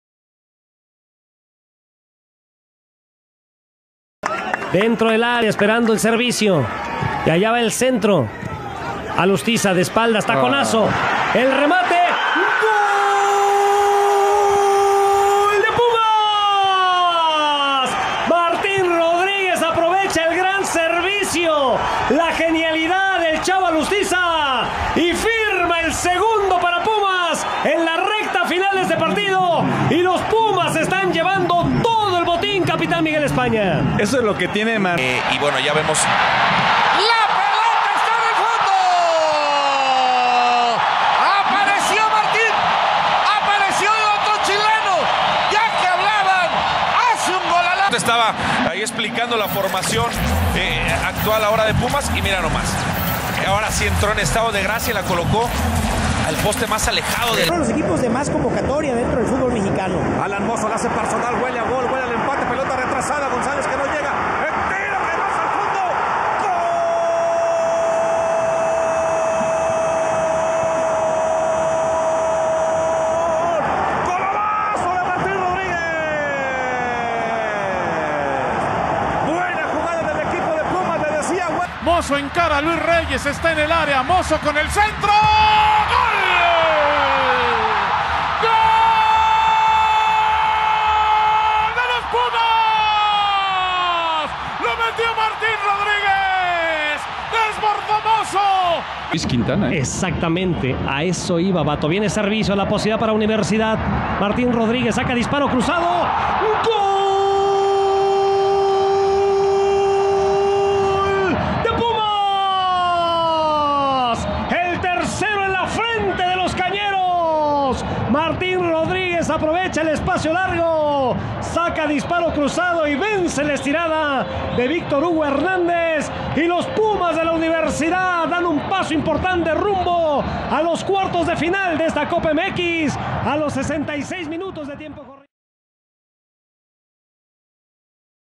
Los 5 Goles de Martín Rodriguez con Pumas
Estos fueron los 5 goles que anoto Martin Rodríguez en Pumas.
Nuevo refuerzo del Morelia.

Año de Nacimiento: 1994 (Chile)
Posición: Medio Izquierdo
Partidos Jugados: 52
Tiempo en Pumas: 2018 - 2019
Títulos con Pumas: 0

Goles:
1. Vs Tampico (Copa MX AP2018)
2. Vs Necaxa (Liga MX AP2018)
3. Vs Guadalajara (Liga MX AP2018)
4. Vs América (Liga MX AP2018)
5. Vs Zacatepec (Copa MX CL2019)